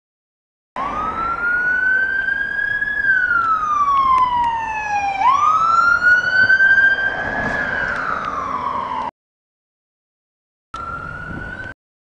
Yorkshire Ambulance Service - Mercedes Sprinter On Shout
Another Mercedes Sprinter on Shout Down Hessle Road